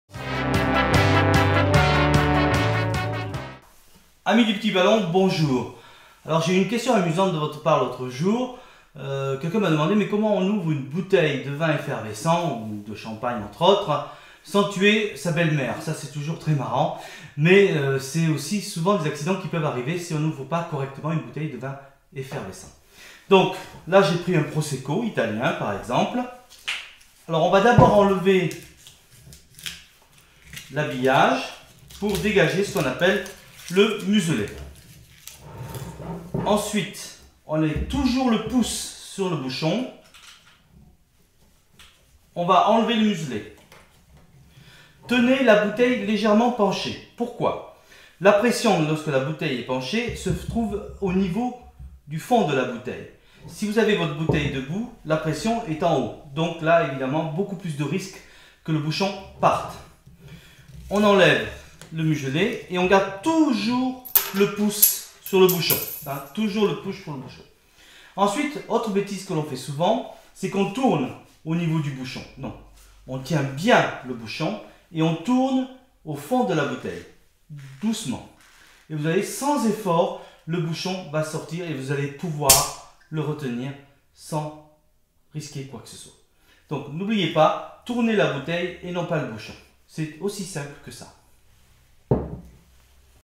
Comment ouvrir une bouteille de vin effervescent ? | Les astuces du Petit Ballon
Ouvrir une bouteille de vin effervescent vous effraie ? Découvrez les astuces de Jean-Michel Deluc. L'abus d'alcool est dangereux pour la santé. Sachez consommer avec modération.